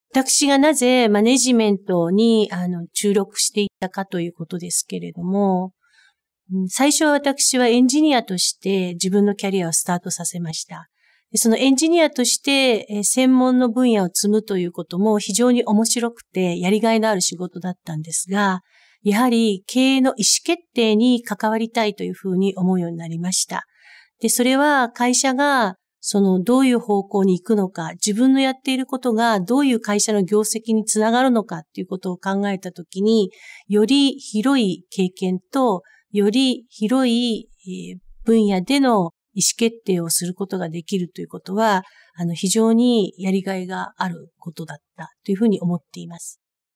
Why Management (Japanese) | Head of Government Affairs Takako Ebata | Career Girls
Watch Takako Ebata, Head of Governmental Affairs & Policy at Johnson & Johnson in Japan. In her job, she builds the network to shape the environment for their policy in the healthcare industry.